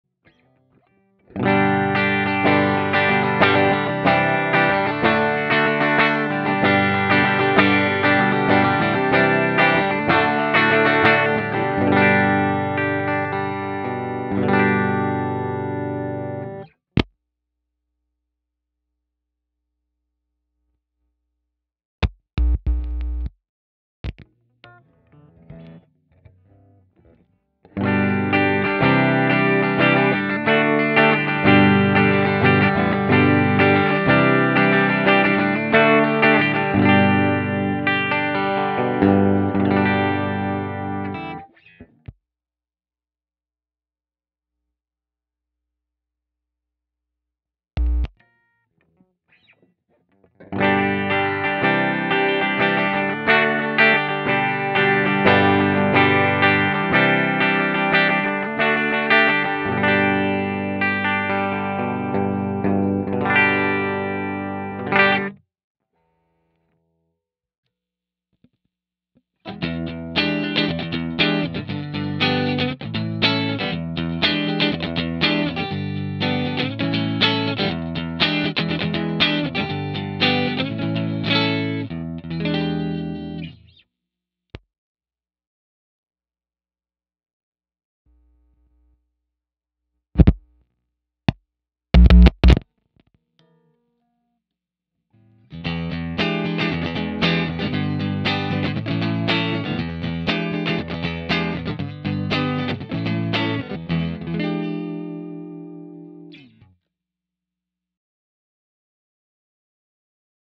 Guitar Sound Test - Body Material Comparison - Alder vs Basswood vs Acrylic FULL VIDEO NO EDIT
3 Guitars with same pickup - Seymour Duncan SH-4 JB Bridge

1 - EVH VH-1 Strat - Alder 2 piece Body
2 - Aria Pro Excel - Basswood multi ply wood ( lamineted )
3 - Kramer Bareta 422 - Acrylic 1 piece body

Bonus - Kramer with SSL-1 vs Fender with SSL-1

M-Audio ProFire 610
GuitarRig 5
Jazz preset, clean, no effects, no compressor.